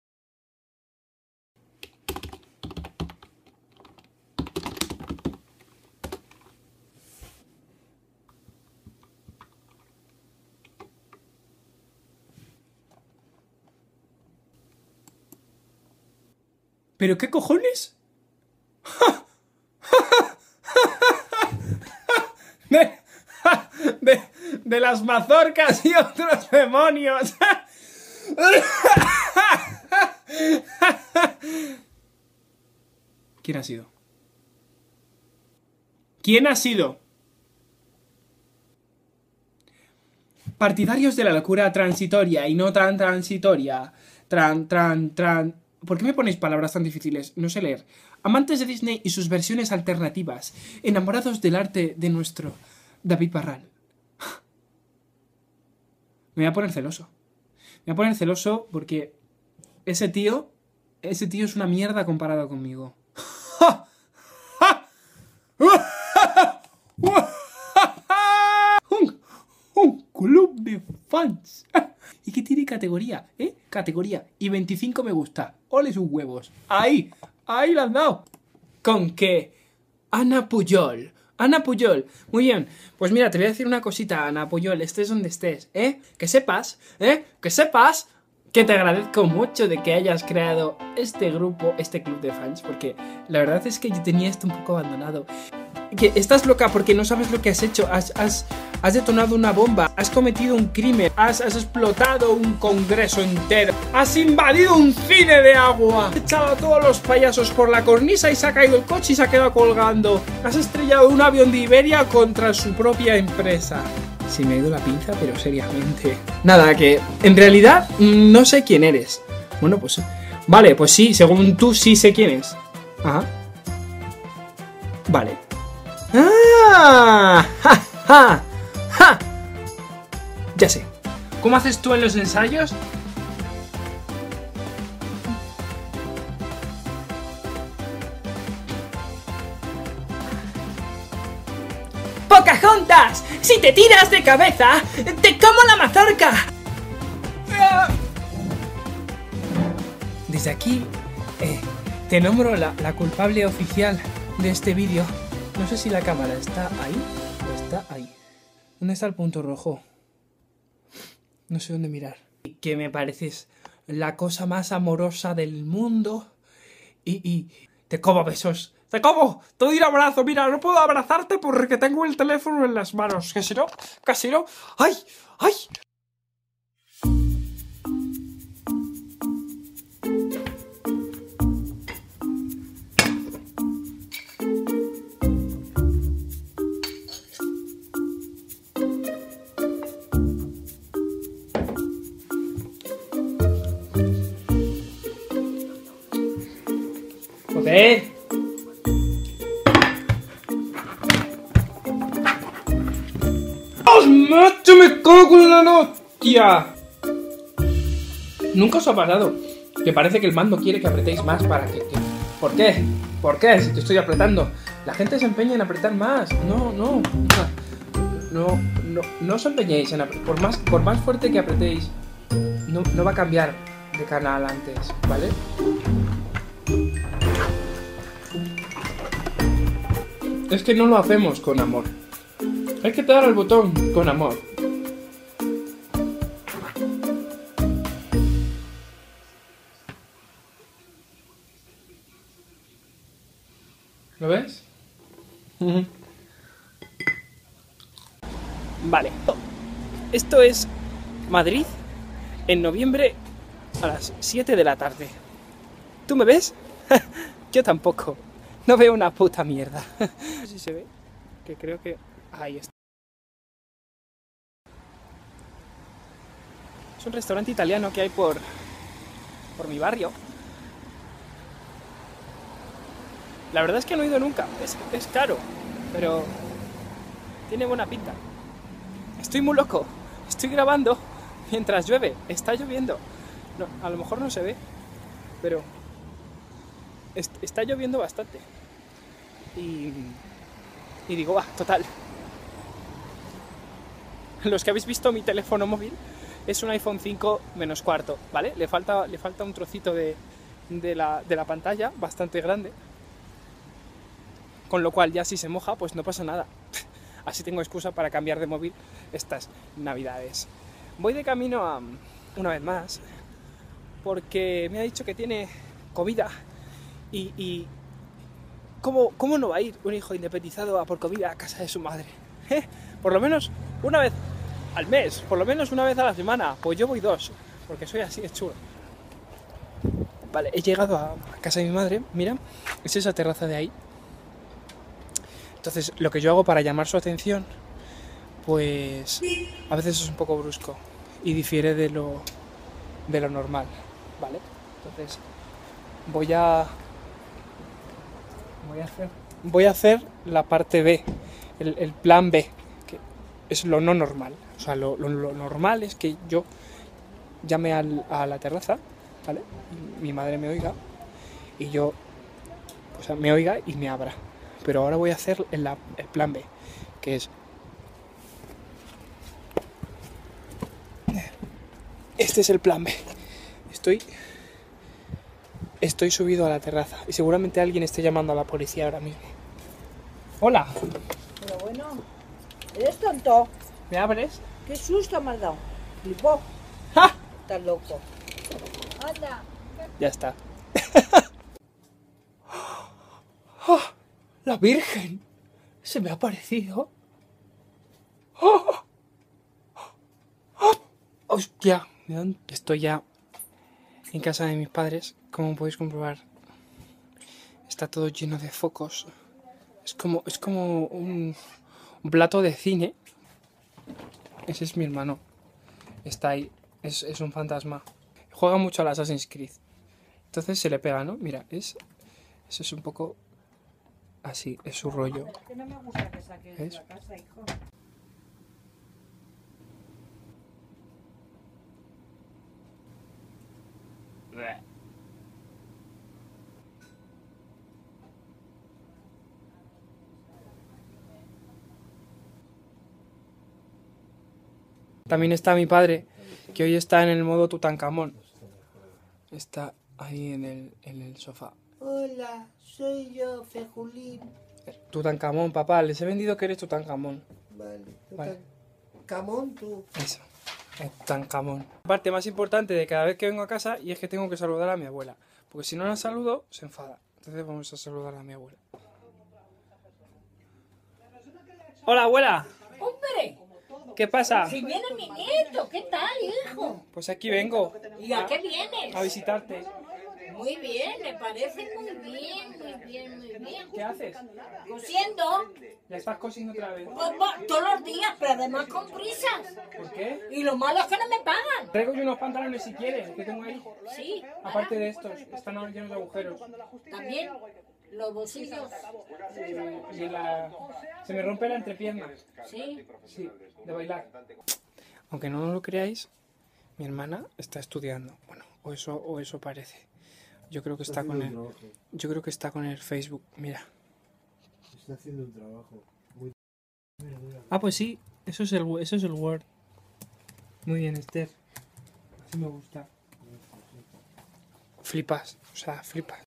UveVlog 8 - El club de fans
Penes a todos!

¿No sabeis como tratar a un mando de distancia?
¿Quereis aprender a grabar con la cámara frontal y en baja calidad?
¿Quereis conocer a mi familia y dejar a mi madre para otro día porque resulta demasiado agresiva?

Entonces este es vuestro video. Espero que os guste. He vuelto =)